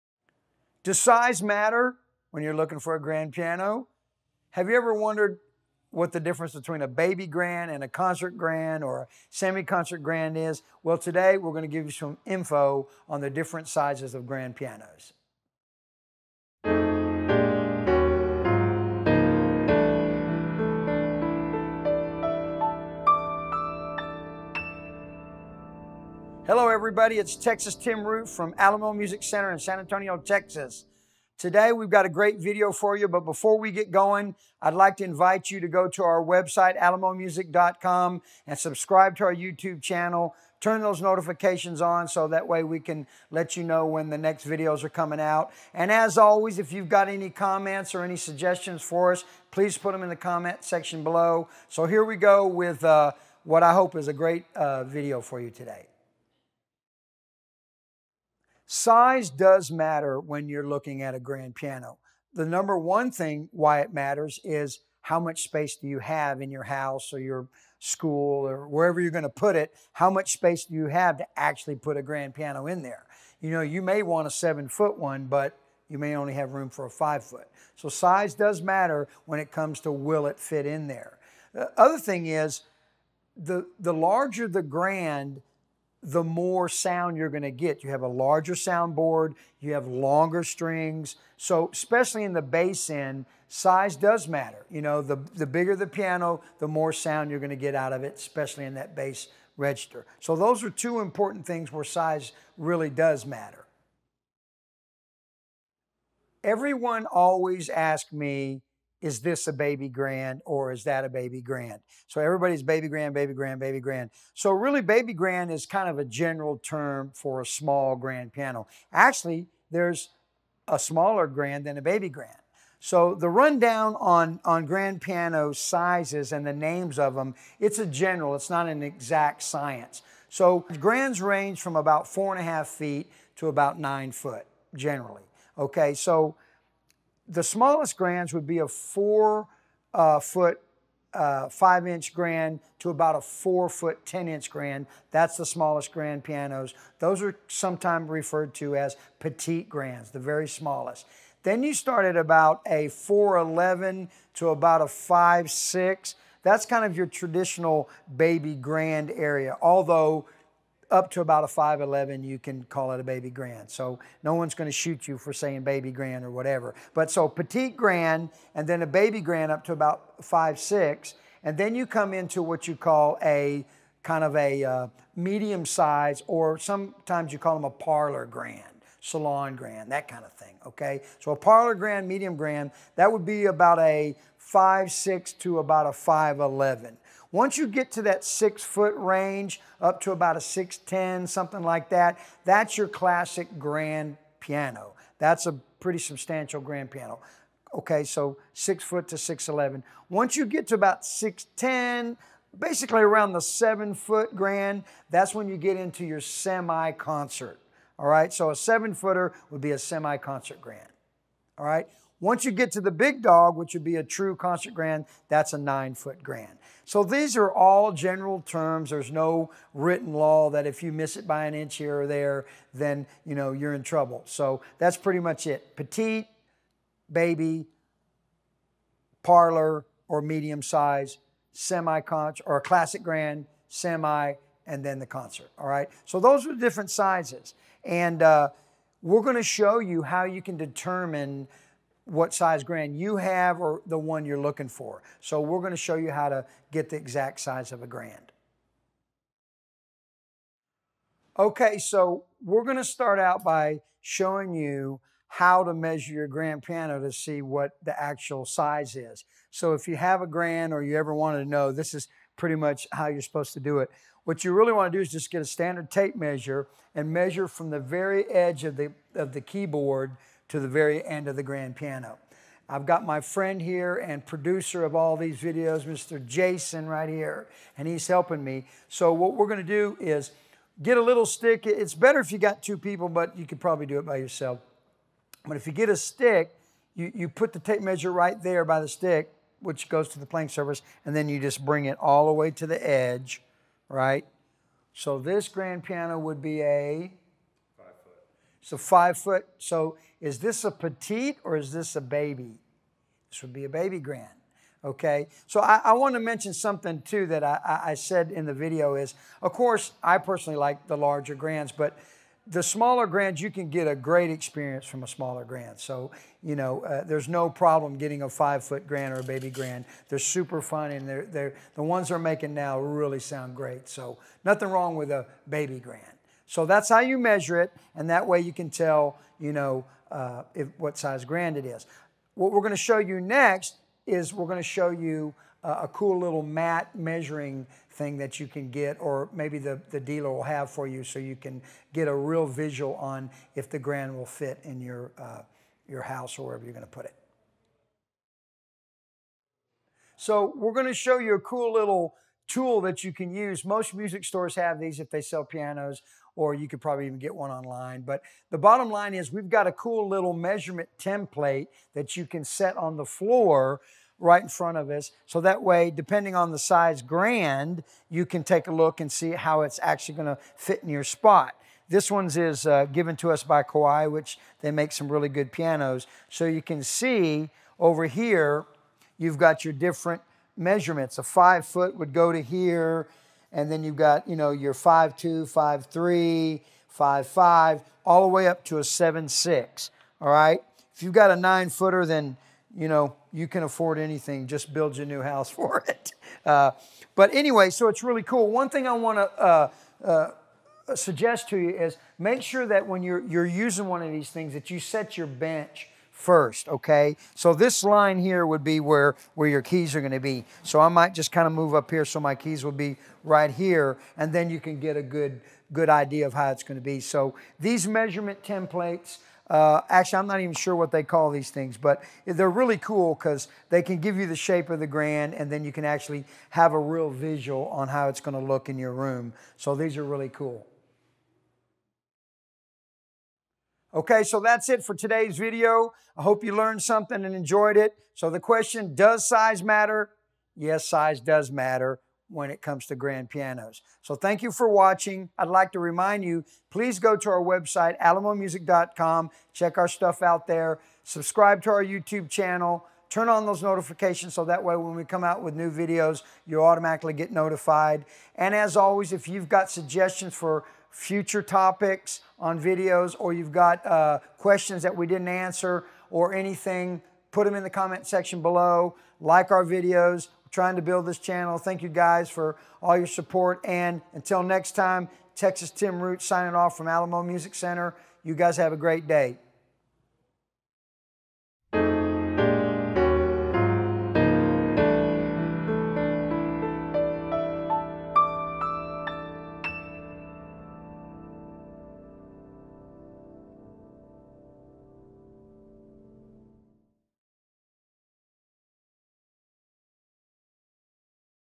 Does Size Matter? Finding The Right Size Grand Piano For You
Okay, you have decided to get a grand piano, but what size?  There are a few important factors that determine what size grand piano is right for you.